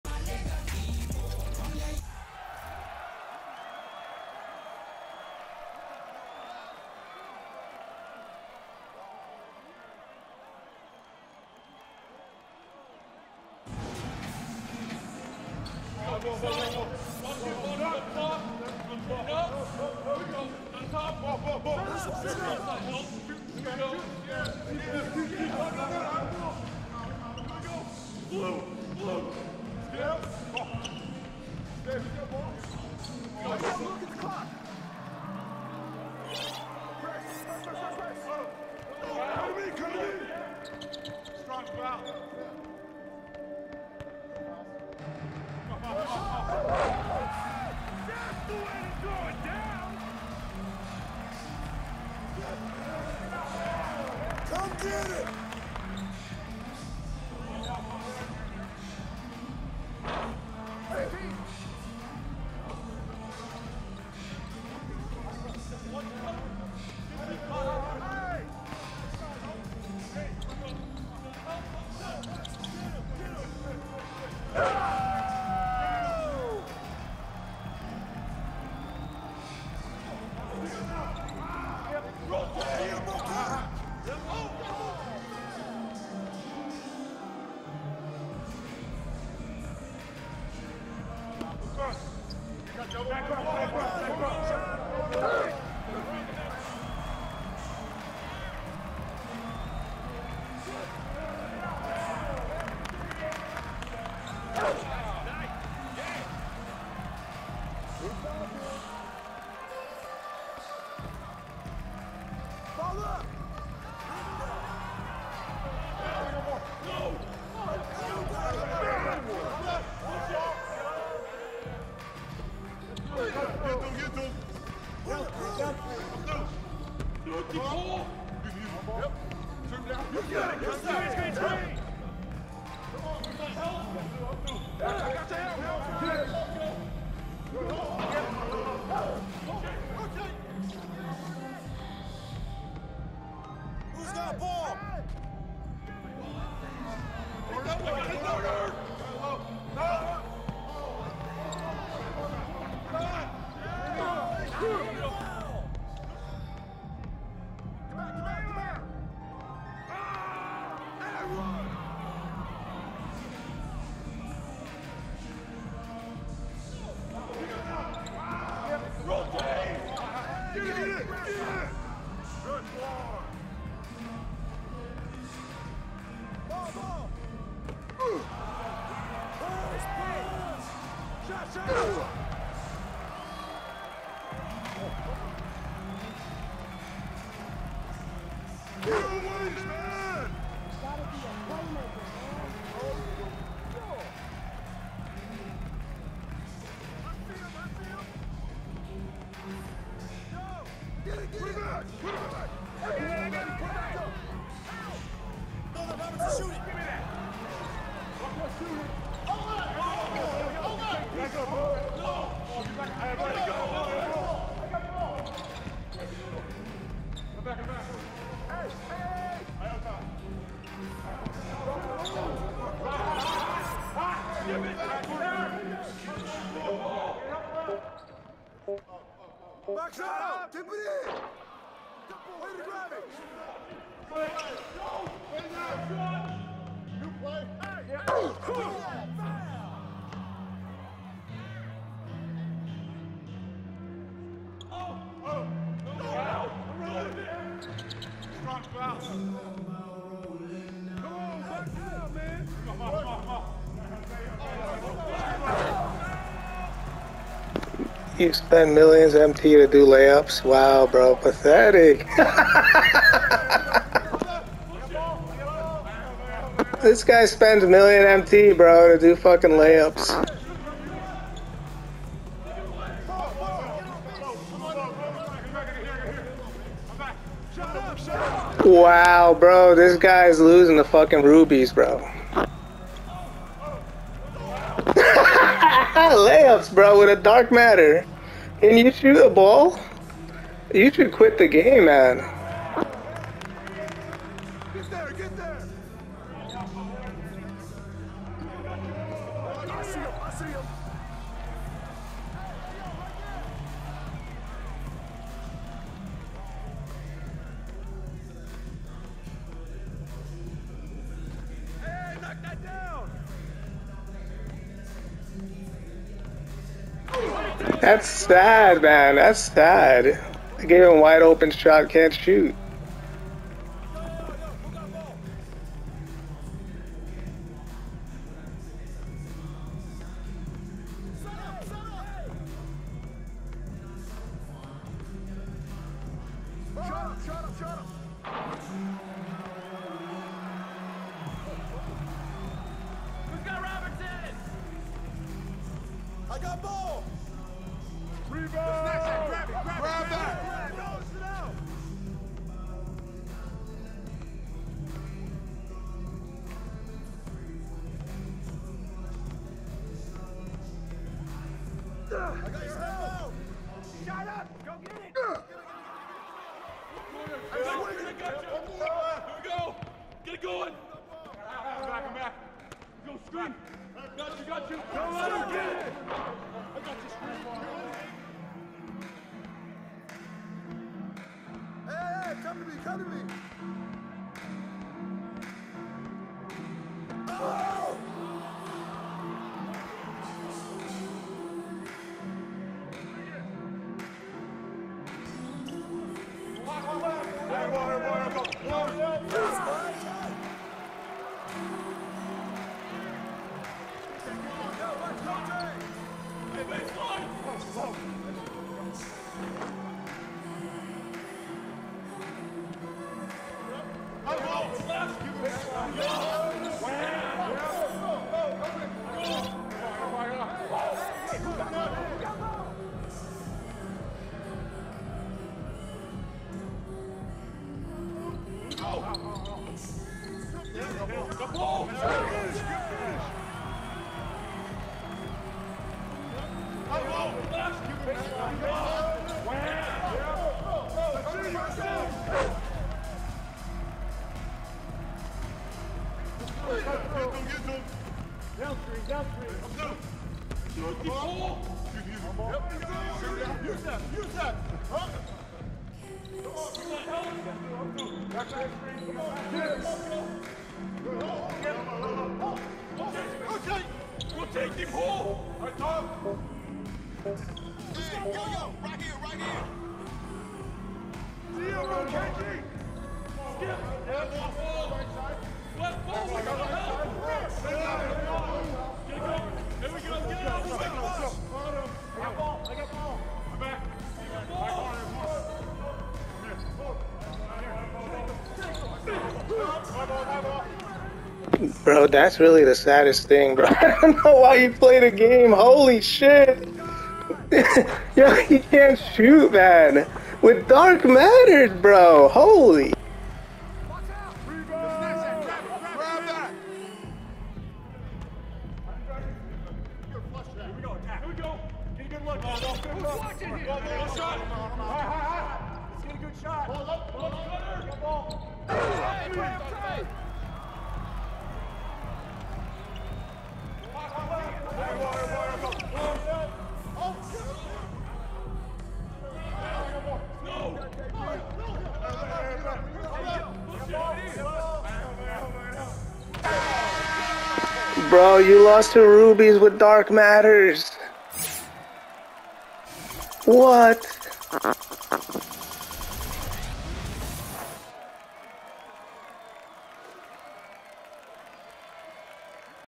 Did I beat a 99 overall Dark Matter team Bosh Oscar KG with my ruby budget team?
Proof you don't need to spend all your money to win